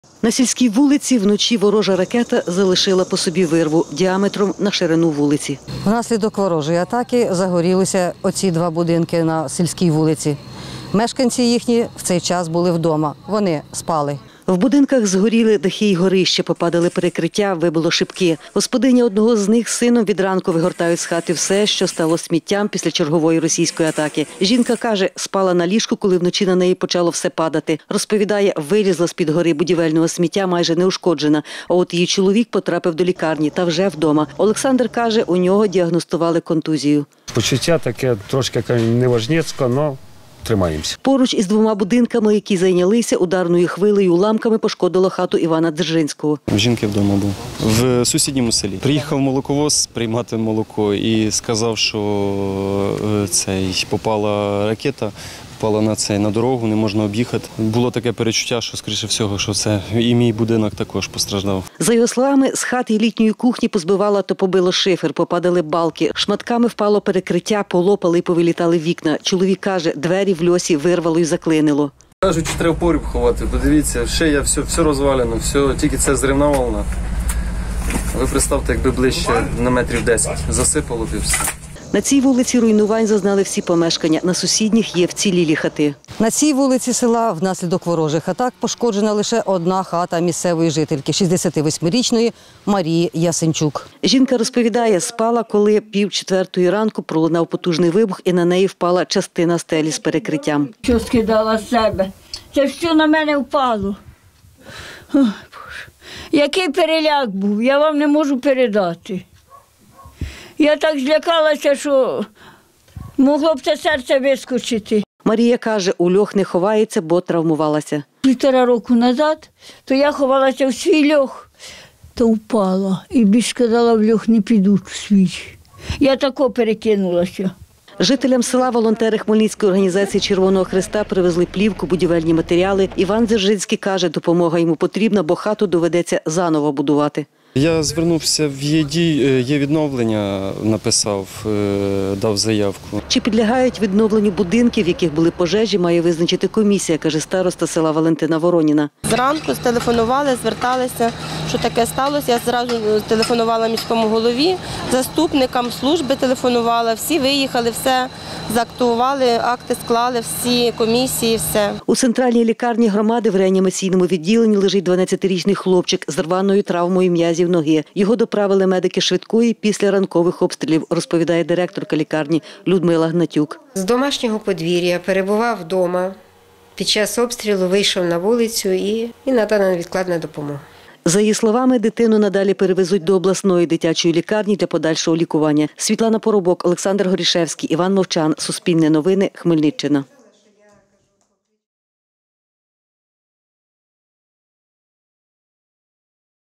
Наслідки російської атаки на Хмельниччину
Два будинки погоріли після нічних обстрілів у селі Хмельницького району-розповідає староста села Валентина Вороніна. За її словами, господарі лишилися живі, хоча й не без ушкоджень, а от домівки та господарські споруди напівзруйновані і можуть не витримати ремонту.  Під час ранкової російської атаки поранення дістав 12-річний хлопчик.